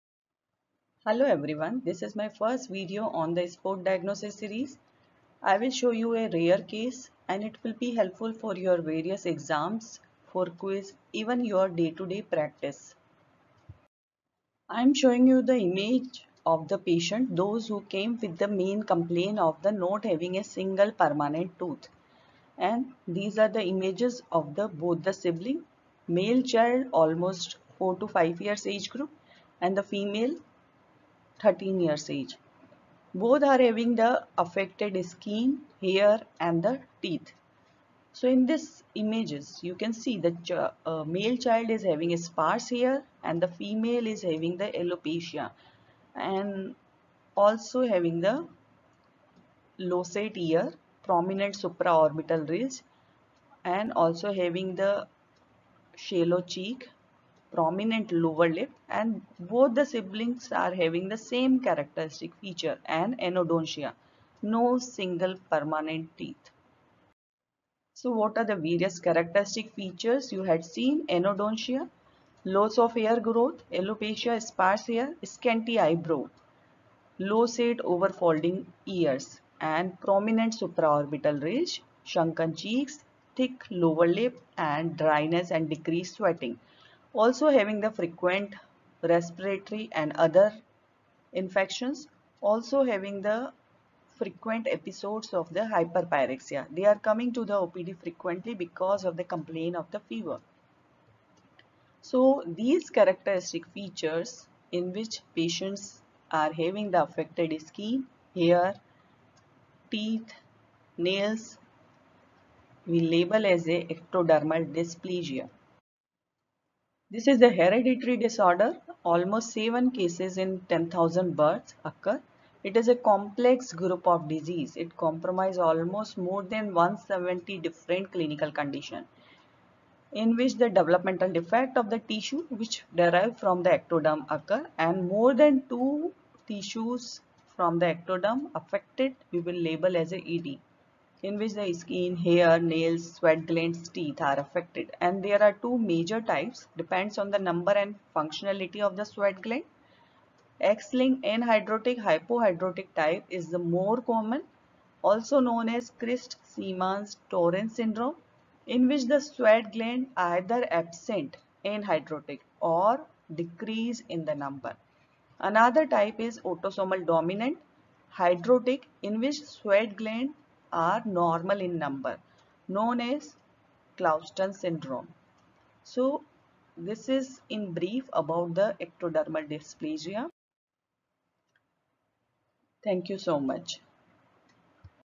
Ectodermal Dysplasia || Spot Diagnosis - 1 || Pediatrics
This videos describes the clinical features of a rare case of Ectodermal Dysplasia.